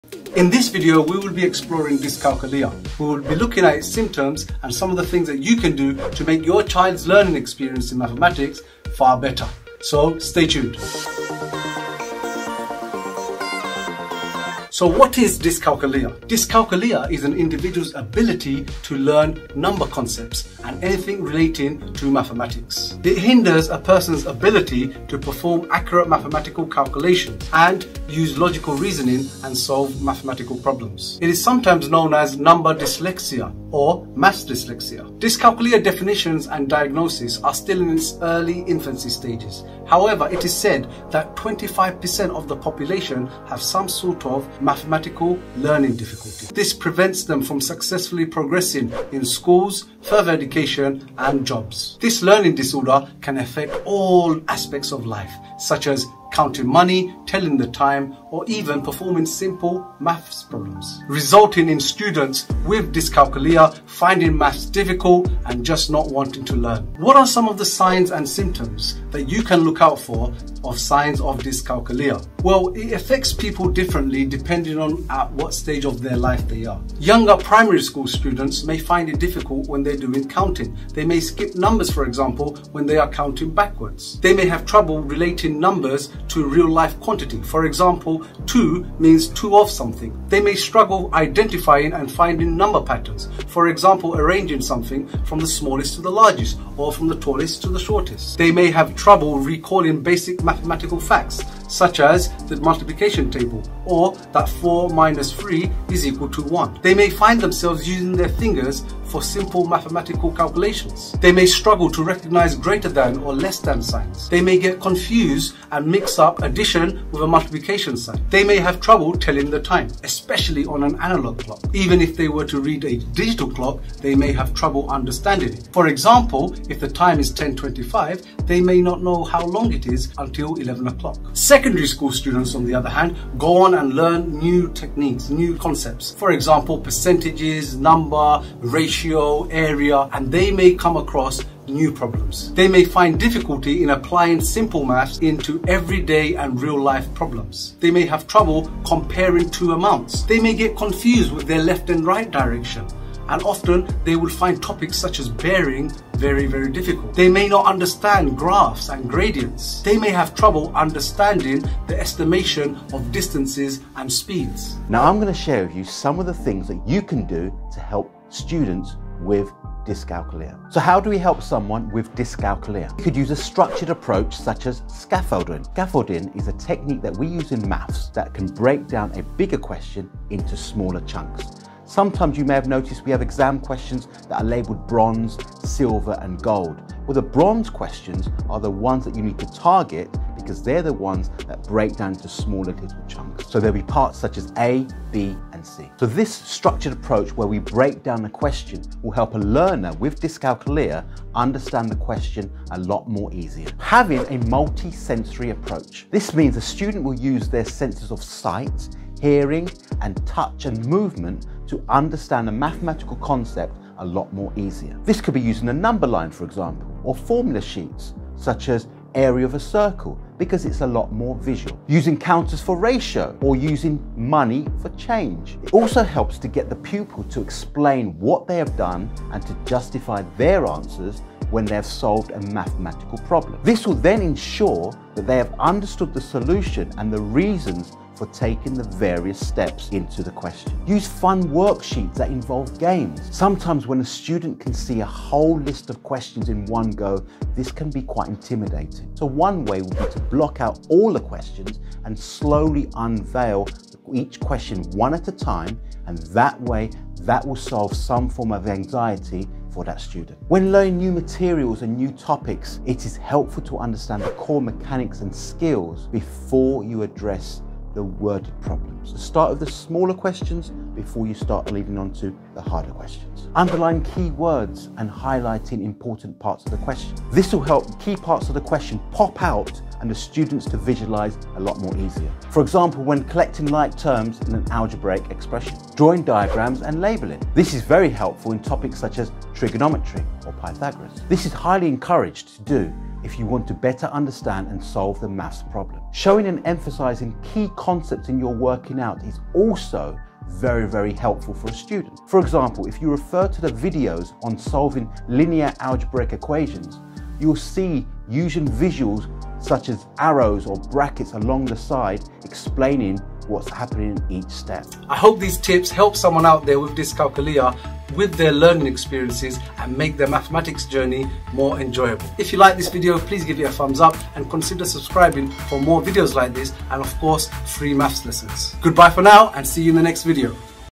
What is Dyscalculia? The Maths Dyslexia - Symptoms, Solutions & Tips
You may not be familiar with Dyscalculia however, this mathematics learning difficulty may be quite common in students and even adults.
The Maths Dyslexia refers to a wide range of lifelong learning disabilities involving maths and whilst research and symptoms are in its early infancy stages, it is said that up to 25% of the population have some form of Maths Learning Difficulty.
If addressed early, you can make your child's learning experience in Maths Lesson one which is much more enjoyable and productive.
I will share with you tips andsolutions to help manage the issues relating to Maths Dyslexia (Dysclaculia)

00:00:00 Intro
00:00:15 What is Dyscalculia ?
00:00:31 The Maths Dyslexia
00:00:48 How Dyscalculia affects people
00:01:07 What are the signs and symptoms of Dyscalculia?
00:01:17 Effect on Primary school pupils
00:02:14 Effects on Secondary Pupils
00:02:45 Solutions and Tips to manage Dyscalculia
00:05:15 Outro

🔴 For GCSE Further Maths Topics  🔴

🐣Serving you so that you get the grade that you deserve  👊🏽